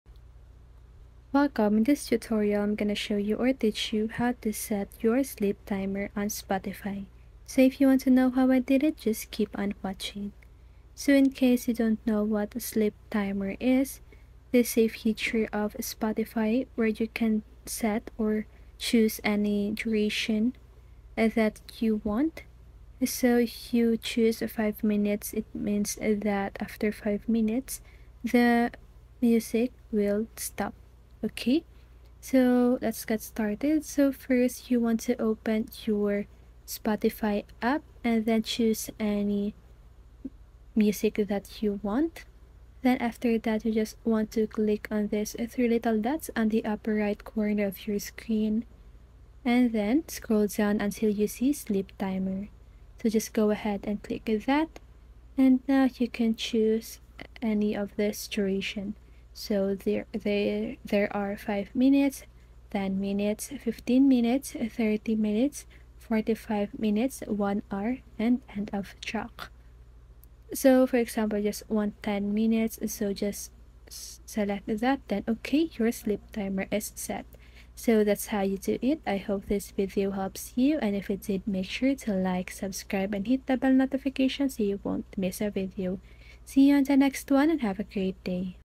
How To Set Your Sleep Timer on Spotify (2023 Update) | Turn on Sleep Timer Spotify
How To Set Your Sleep Timer on Spotify (2023 Update) | Turn on Sleep Timer Spotify. In this video you will learn how to turn on sleep timer for Spotify. Step by step instructions on how to set sleep timer on Spotify. Learn how to enable sleep timer for Spotify with the new 2023 update. 💭 Have a question about this tutorial?